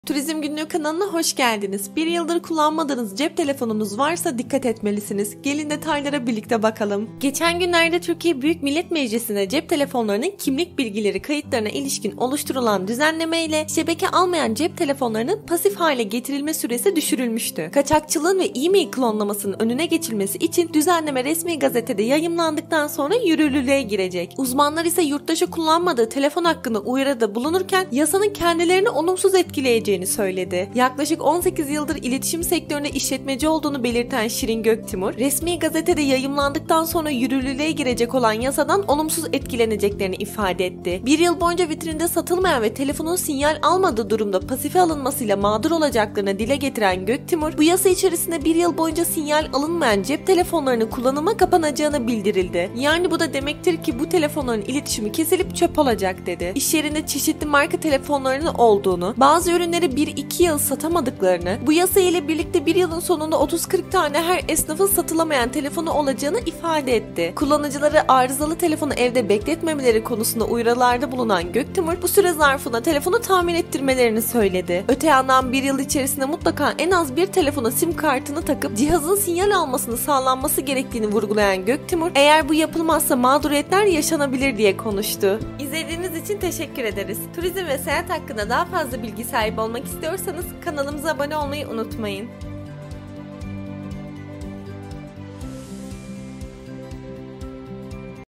1 YILDIR KULLANILMAYAN CEP TELEFONLARI ÇÖP OLACAK! / YAPMANIZ GEREKENLER | TURİZM GÜNLÜĞÜ
Elektronik haberleşme şebekelerinden hizmet almayan telefonların IMEI bilgilerinin pasife alınması, 7 yıldan 1 yıla düşürülüyor.

Düzenleme Resmi Gazete’de yayımlanmasıyla birlikte yürürlüğe girmiş olacak. Telefoncular bu konuda tüketicileri uyardı.

Geçen günlerde TBMM’de cep telefonlarının kimlik bilgileri (IMEI) kayıtlarına ilişkin oluşturulan düzenlemeyle şebeke almayan cep telefonlarının pasif hale getirilme süresi düşürülmüştü.

Kaçakçılığın ve IMEI klonlanmasının önüne geçilmesi için düzenleme Resmi Gazete’de yayımlandıktan sonra yürürlüğe girecek.